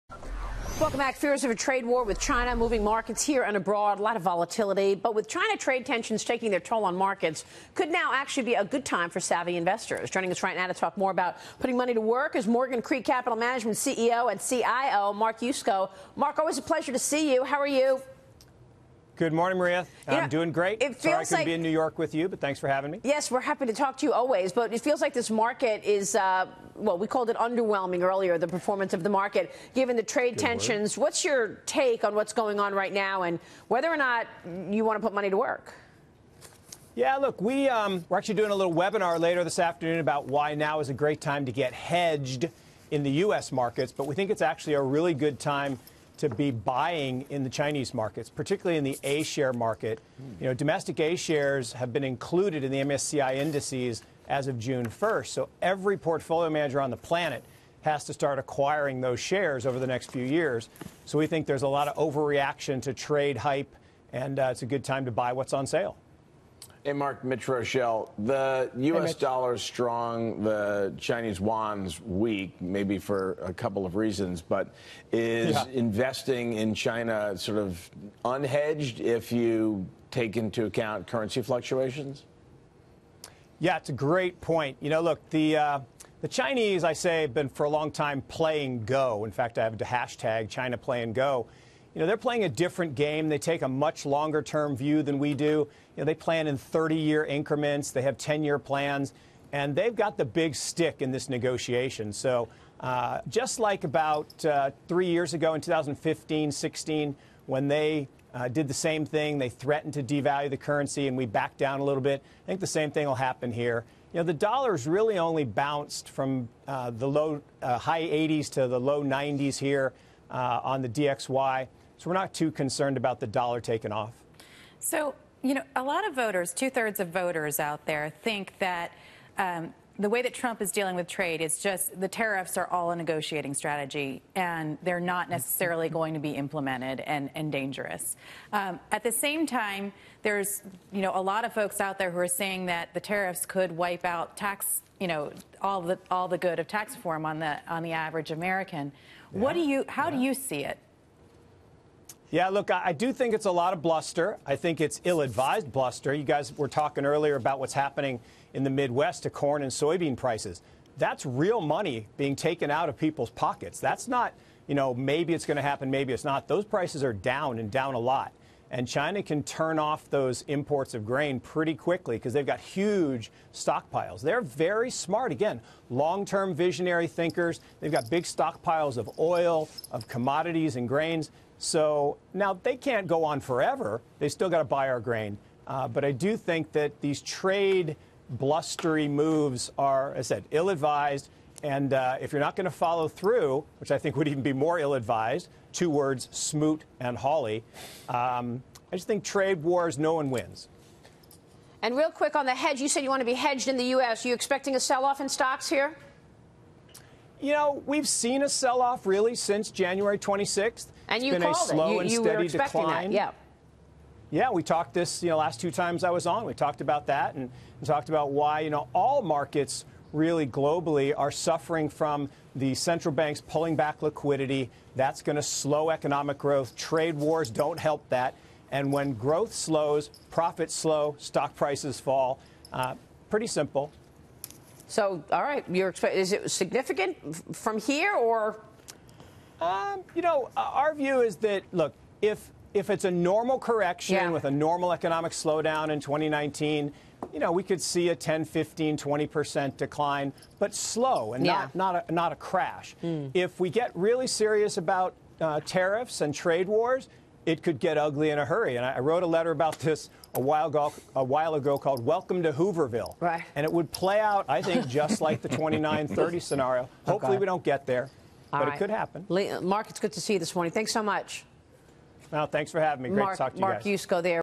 Should investors buy into China despite trade war fears?
Morgan Creek Capital Management CEO Mark Yusko on how investors can benefit from trade tensions with China.